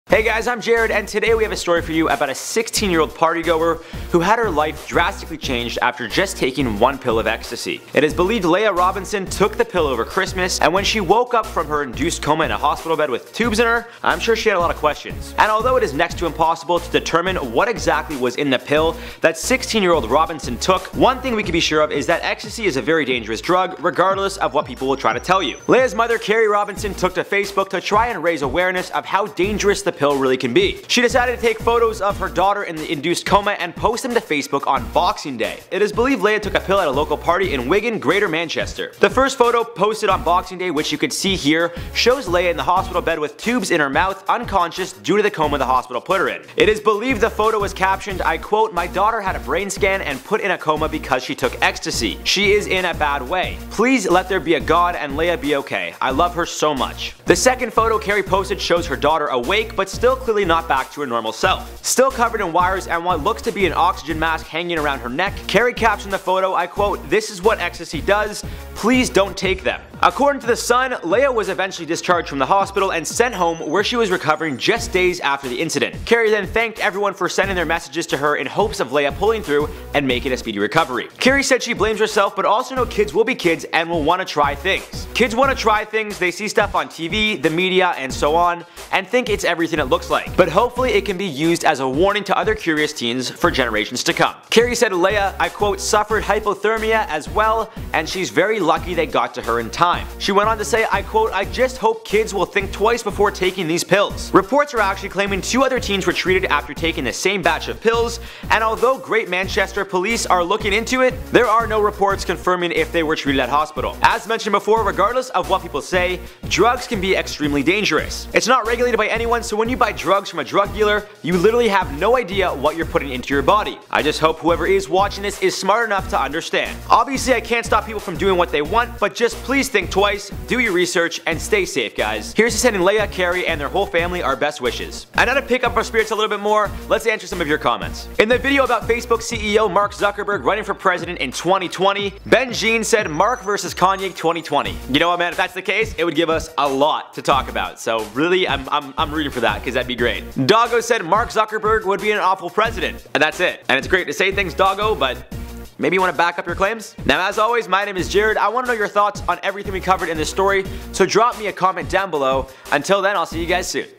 16 Yr Old Girl’s Life Ruined By Party Drug
16 year old Leah Robinson decided to take ecstasy at a party, where the next day, she ended up in the hospital in a coma.  Her mom posted a picture of her daughter on Facebook to teach teens about taking drugs.

2408 Lakeshore Blvd W.